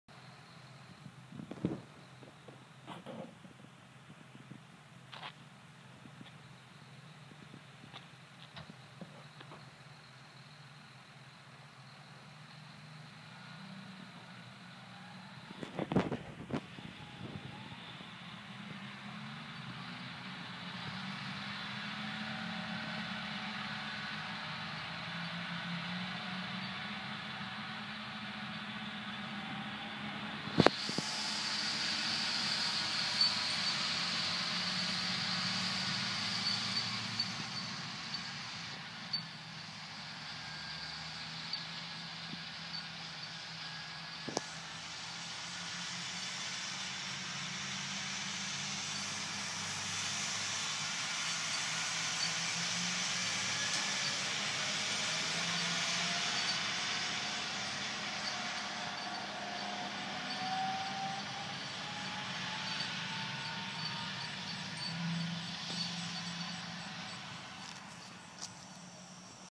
Whole stalk sugarcane harvesting 2019 at my house at 5:25 AM
Just a video of some whole stalk sugarcane harvesting I took at my house